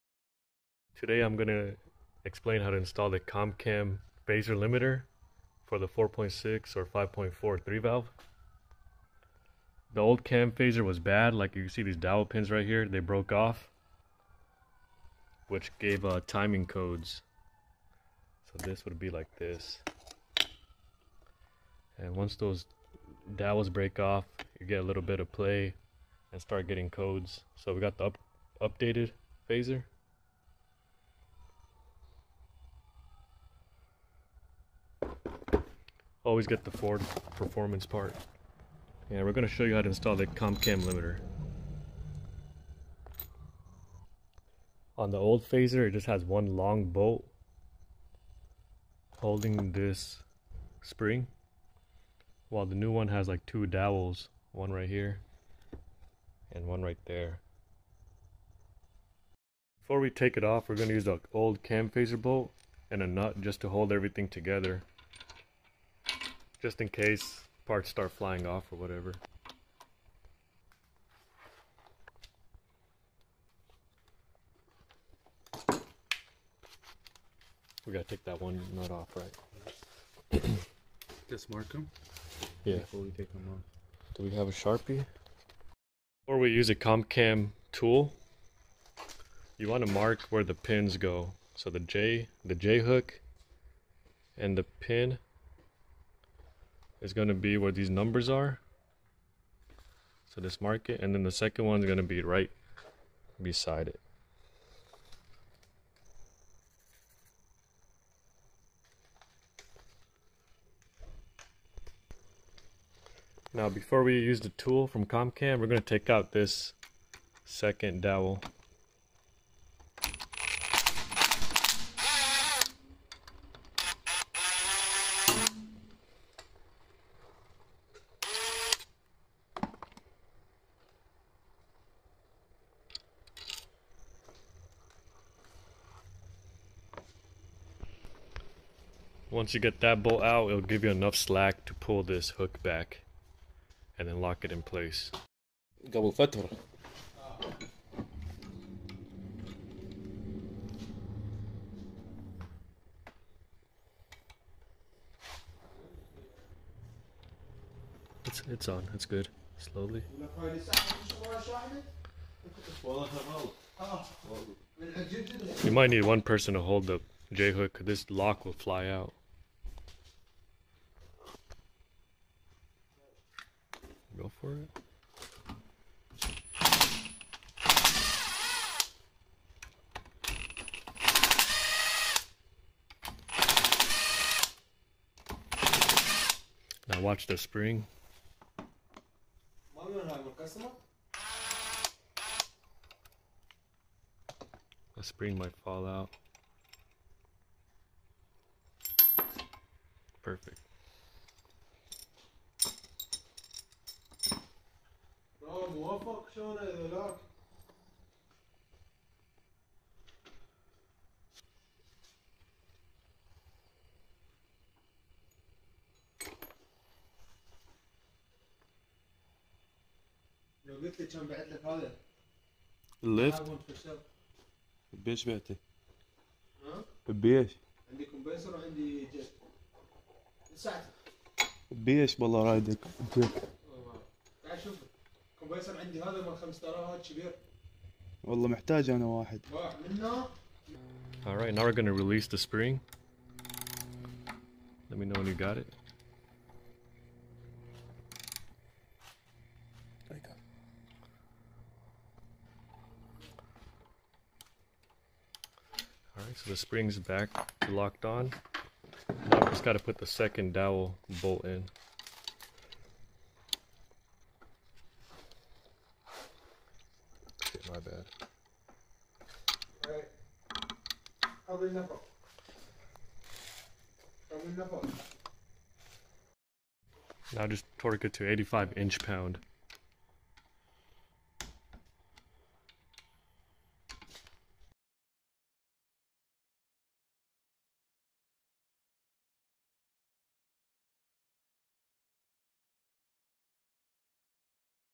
comp cam phaser limiter install 3v CCA-5449
easiest way to install phaser limiters for your 4.6/5.4 3v engine